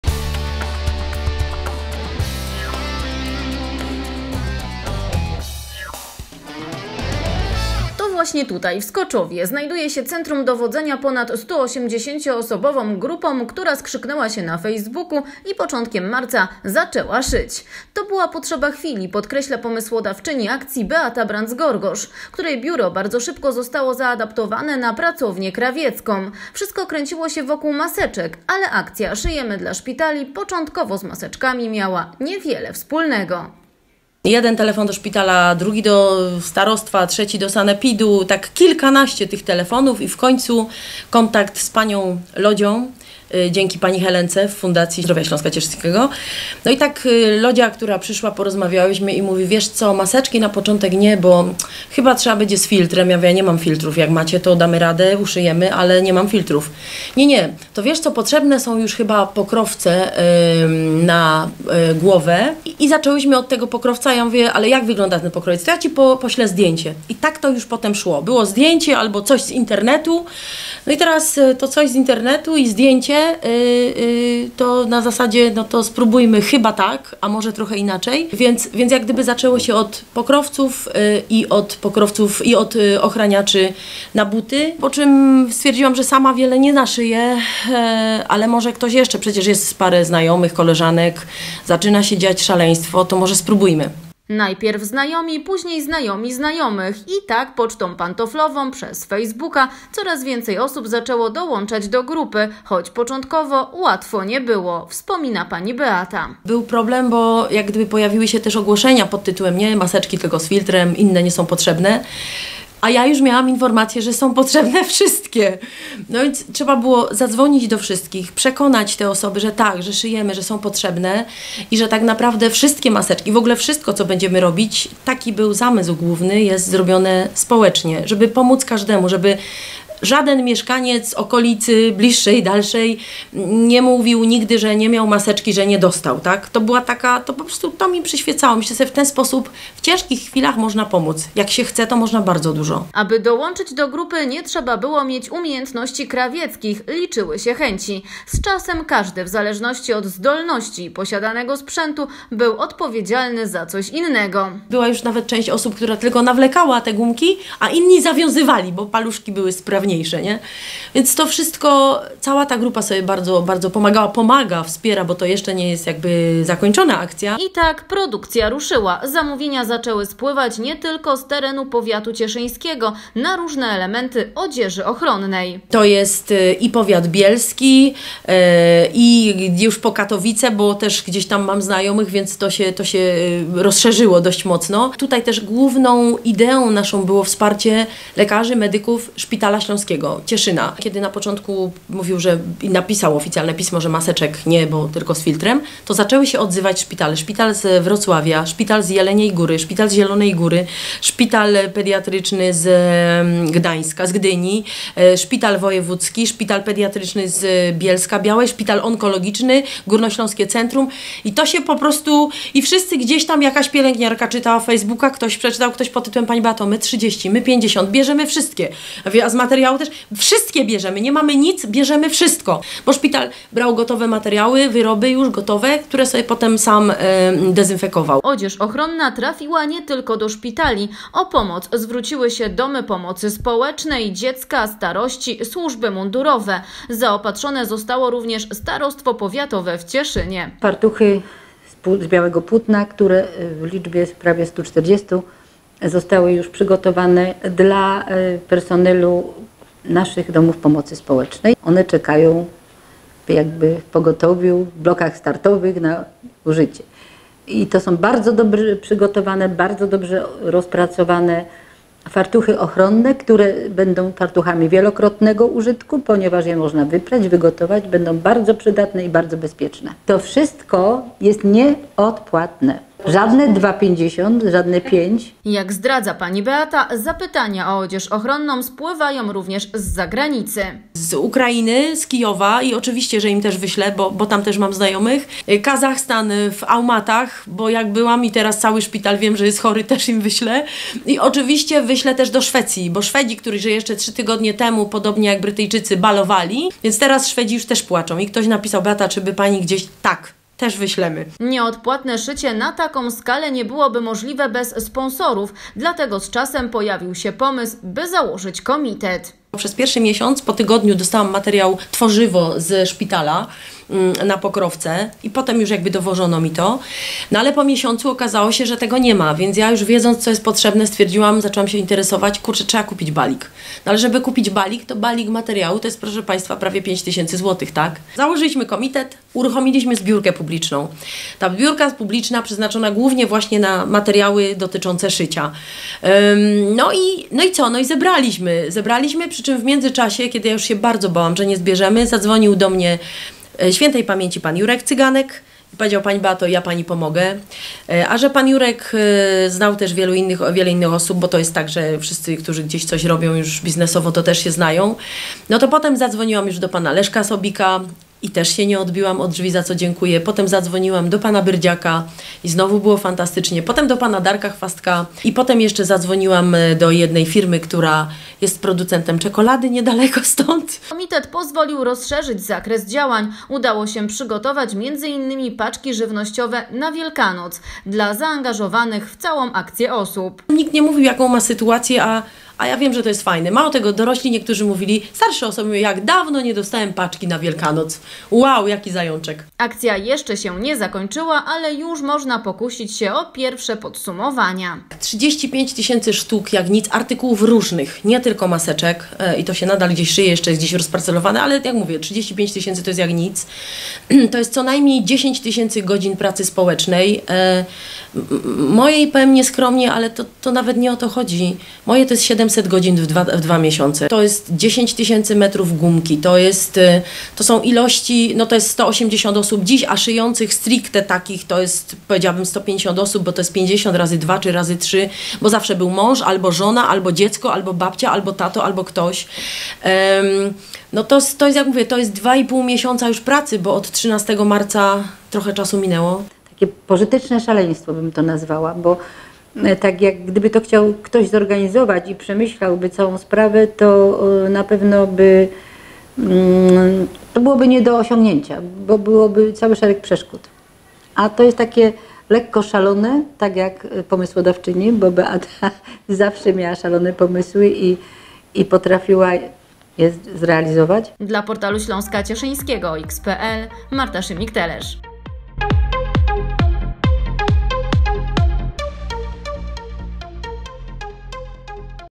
'Szyjemy dla szpitali", czyli 180 osób w akcji
Od 30 maja nie musimy zasłaniać ust i nosa w przestrzeni otwartej. Nie oznacza to jednak, że z maseczkami rozstajemy się na dobre. Jest to jednak dobry czas na podsumowania, bo maseczkowych akcji nie brakowało w całej Polsce, a także i w naszym powiecie. Z kamerą zawitaliśmy do Skoczowa, bo właśnie tam spontaniczna inicjatywa przerodziła się w prężnie działający komitet społeczny „Szyjemy dla szpitali”. Ponad 180 osoba grupa nie zwalnia tempa nawet teraz, ciągle przyjmując zamówienia.